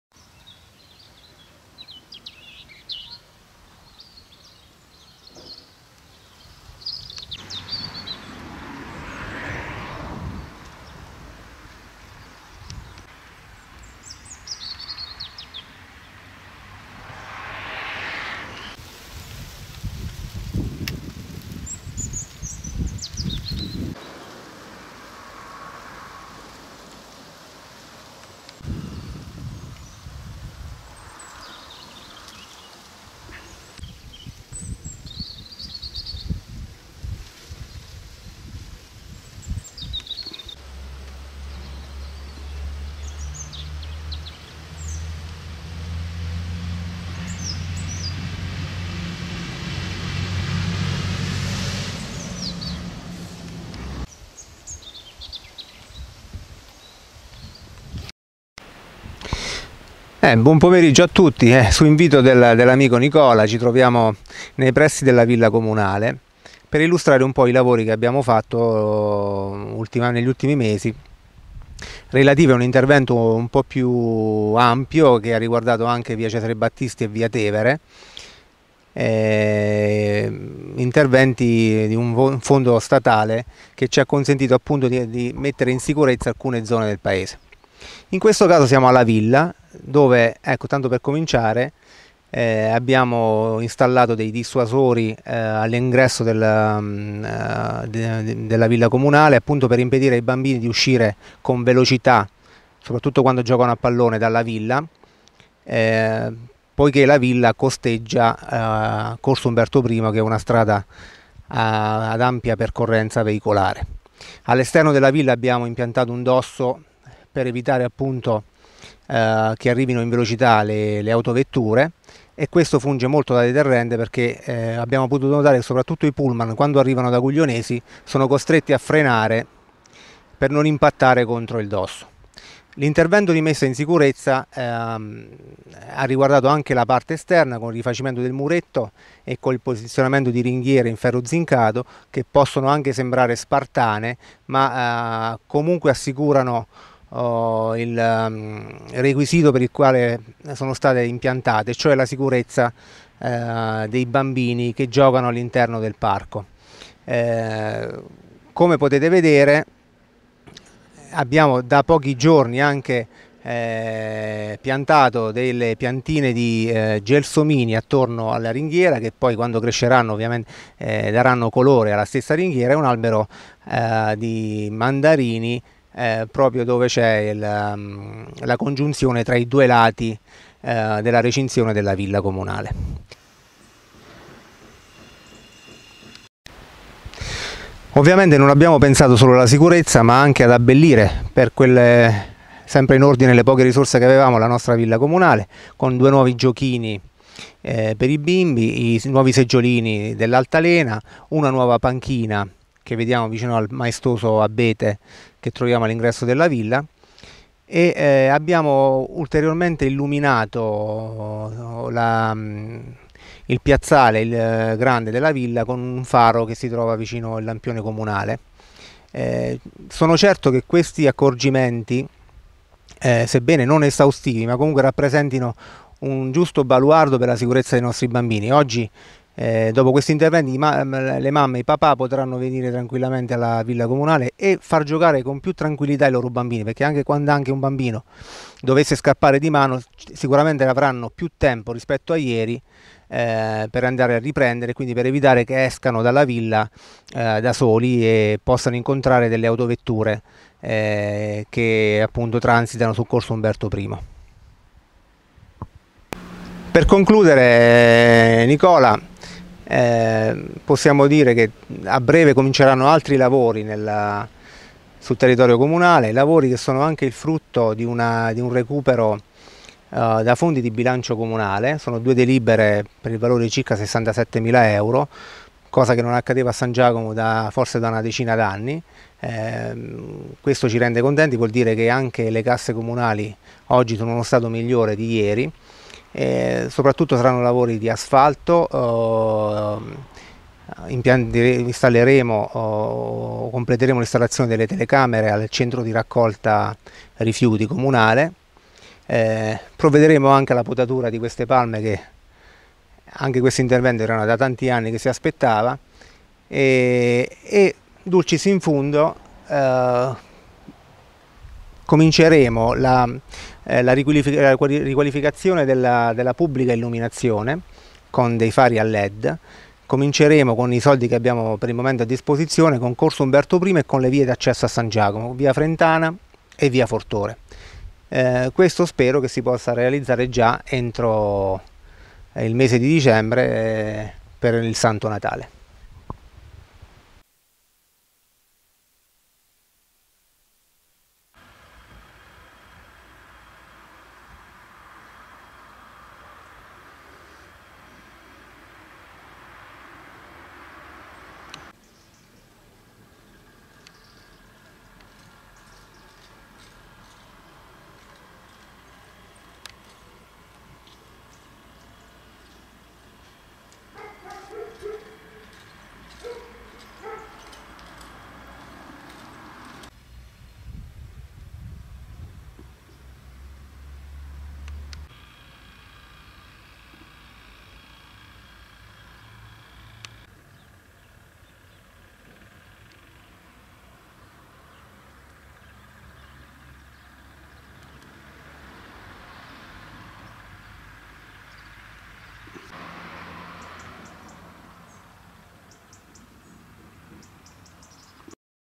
Lavori di riqualificazione della Villa Comunale -HD- 8 novembre 2019 San Giacomo degli Schiavoni -CB
Lavori di riqualificazione e messa in sicurezza della Villa Comunale di San Giacomo degli Schiavoni (CB). 8 novembre 2019 ore 15:00 in Villa Comunale con il sindaco Costanzo Della Porta. Riprese e montaggio di Nicola Mastrogiuseppe.